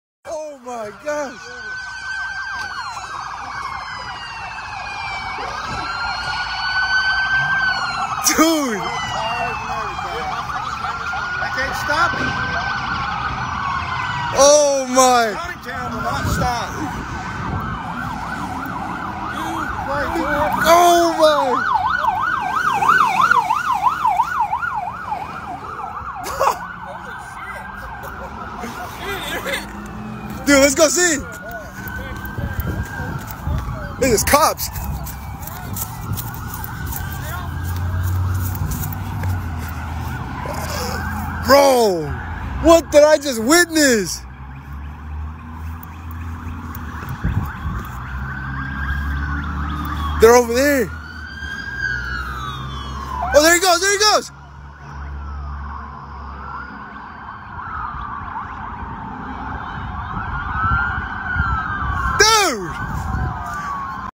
Cop Cars Chase Run Away Van - 1486019
This person heard a lot of sirens and observed several police cars chasing a fleeing van. Cops chased the van across town, speeding after it to prevent the driver from fleeing away.